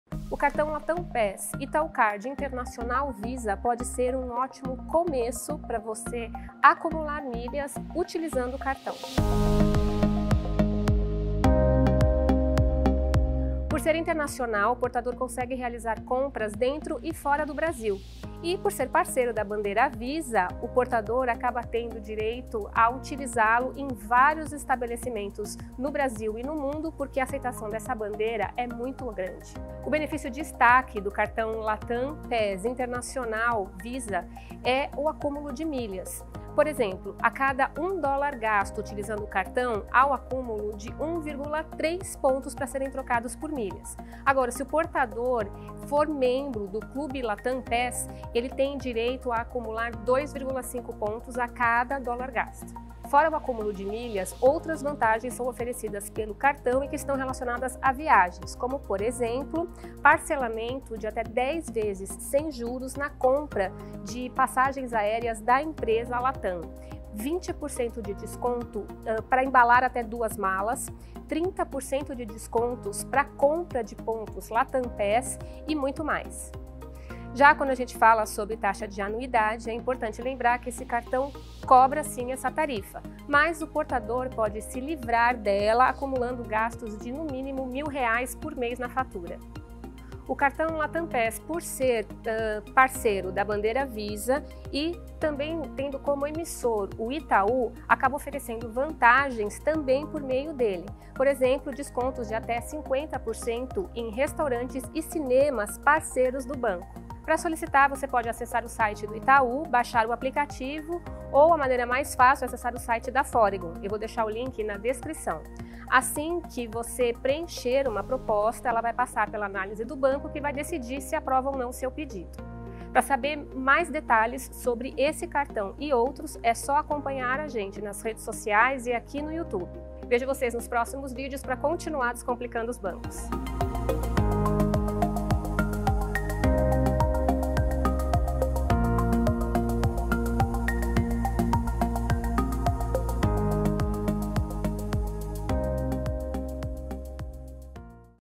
CARTÃO LATAM PASS ITAÚ INTERNACIONAL | FOREGON.COM
Mesmo sendo um cartão de entrada para acumular milhas, o LATAM PASS Internacional Visa pode ser uma boa escolha para começar a pontuar por meio dos seus gastos.

Por ser uma Visa Internacional, ele permite que você faça compras dentro e fora do país, além de ter uma excelente aceitação em milhares de estabelecimentos ao redor do mundo.

Entretanto, o grande benefício do cartão LATAM PASS Internacional Visa está no acúmulo de milhas. Com ele, você ganha  1,3 ponto a cada dólar gasto. Caso você seja um membro do Clube Latam, essa pontuação sobe para 2,5 pontos.

Fora a pontuação, os portadores do cartão LATAM PASS Internacional Visa tem acesso a outras vantagens exclusivas:

Promoções exclusivas;
✅ Passagens aéreas LATAM Airlines em até 10x sem juros;
✅ Limite a partir de R$ 15,00